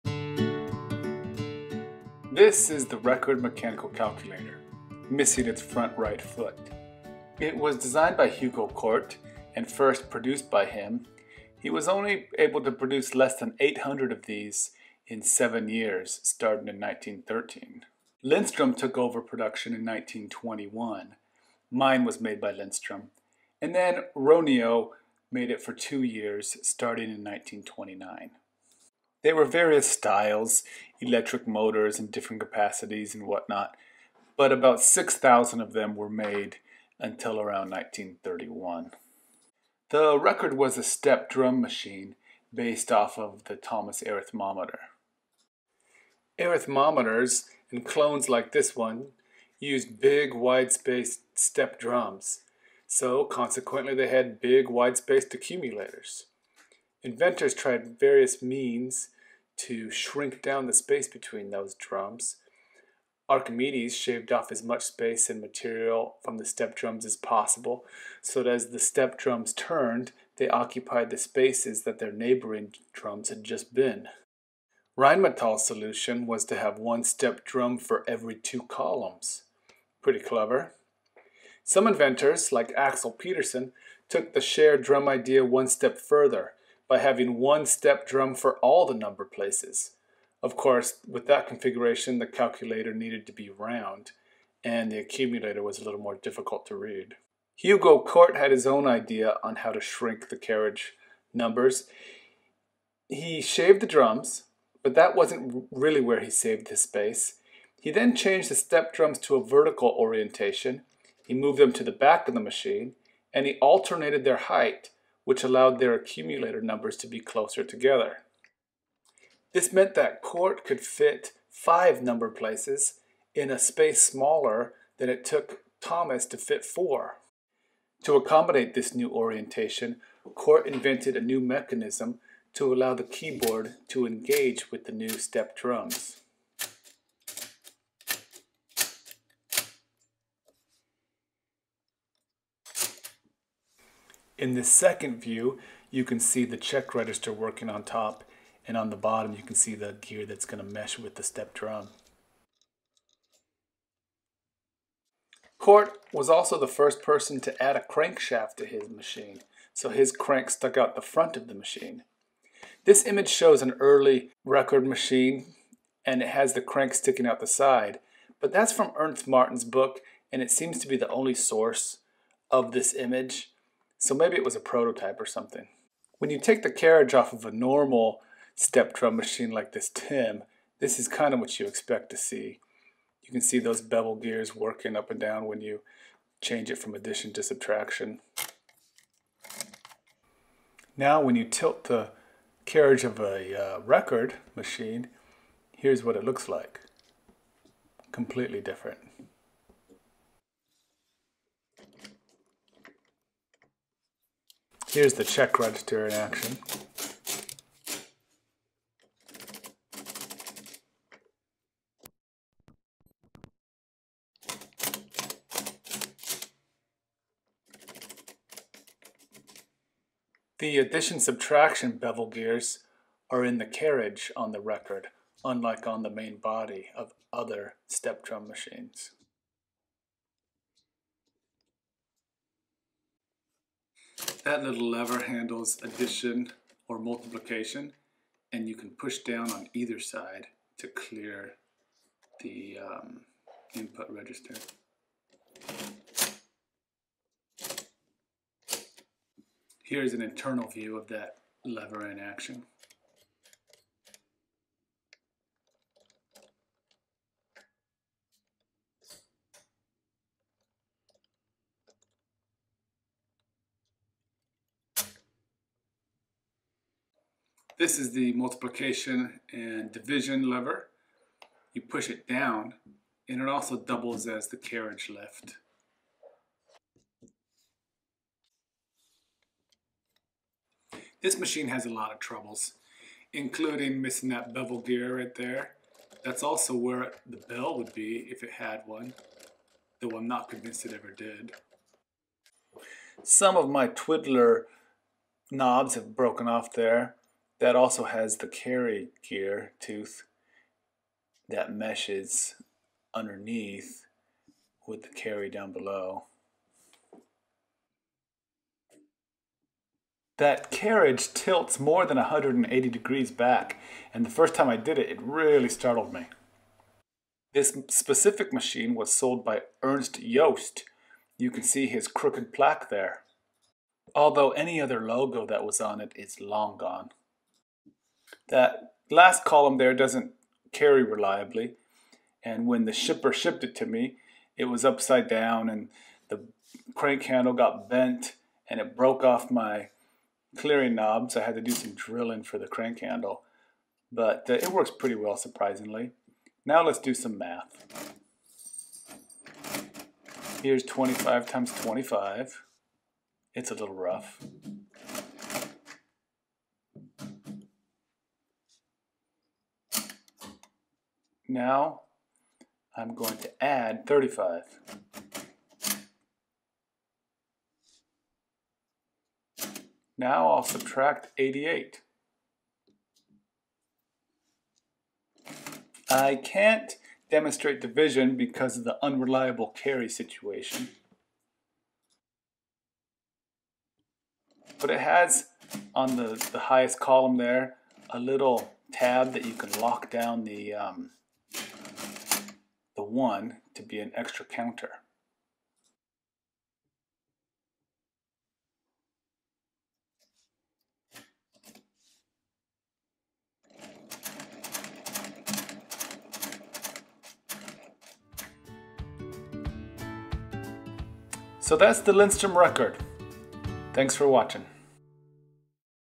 Lindström Record mechanical calculator
It was first called the Record Universal Rechenmaschine by Hugo Cordt then simply “Record” by Lindström. I ordered some knobs to replace the ones that broke during shipping. Shipping damage on these old calculators is so frustrating, especially when the devices don’t come up for sale very often. I got tired of waiting for them to come from China, so I decided to post it without clearing knobs.